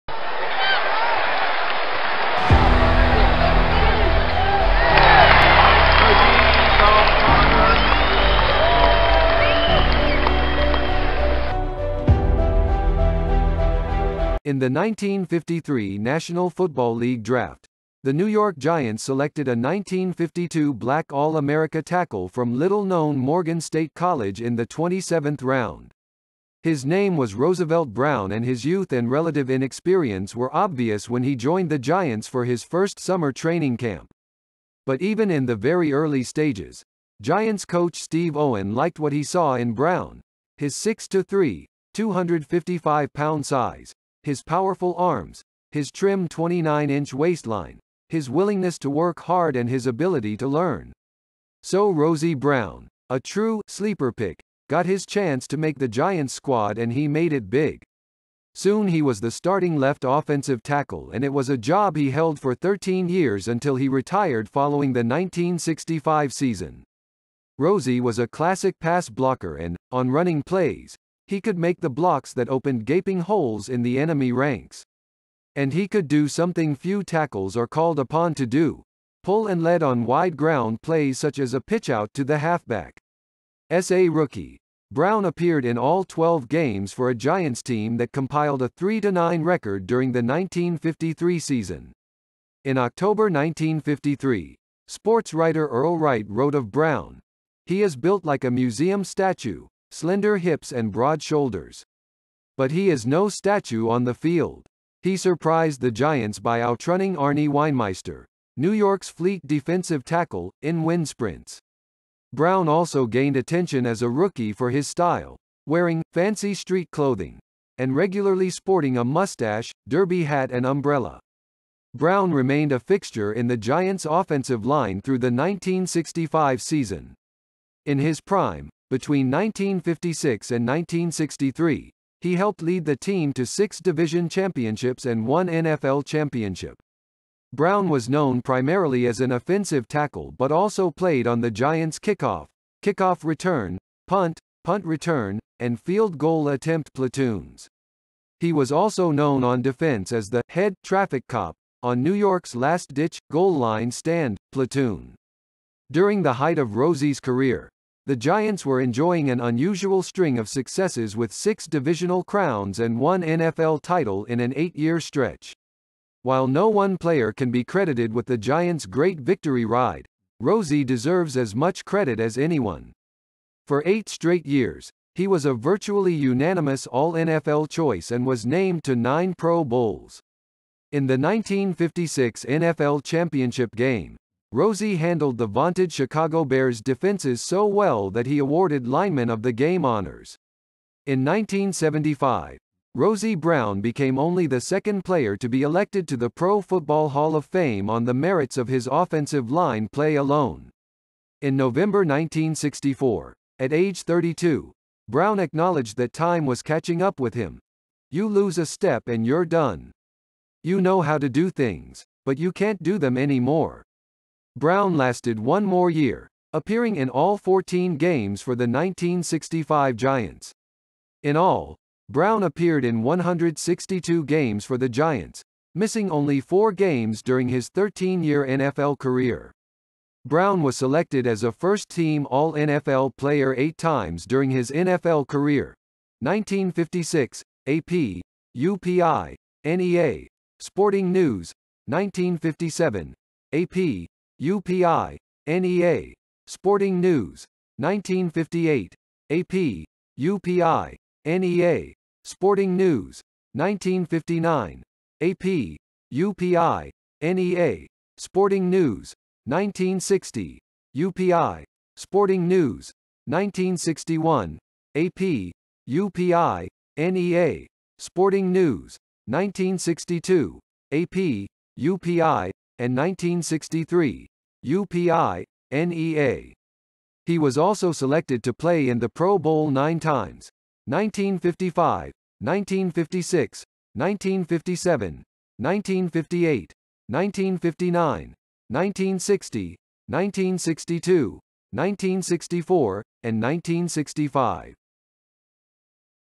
ROOSEVELT BROWN NFL HALL OF FAME CAREER ROOSEVELT BROWN NFL CAREER HIGHLIGHTS
In the 1953 National Football League Draft, the New York Giants selected a 1952 Black All-America tackle from little-known Morgan State College in the 27th round. His name was Roosevelt Brown and his youth and relative inexperience were obvious when he joined the Giants for his first summer training camp.
But even in the very early stages, Giants coach Steve Owen liked what he saw in Brown – his 6-3, 255-pound size, his powerful arms, his trim 29-inch waistline, his willingness to work hard and his ability to learn.

Hello everyone! In Today's video, we show you Roosevelt Brown career highlights. Roosevelt Brown Hall Of Fame Career, Roosevelt Brown Highlights.